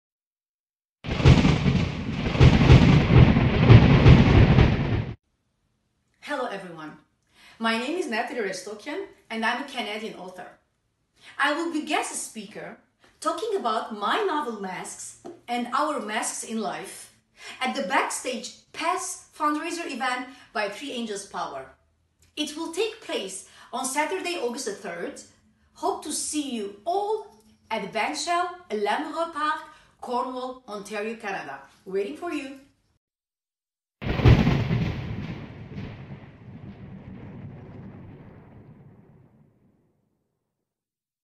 Nataly Restokian Promo by 3AngelsPower
This is a promo for BackStage Pass fundraiser event at the Bandshell (L'amoureux Park), an event that will take place on August 3rd 2019 from 3:30PM til 11:00PM.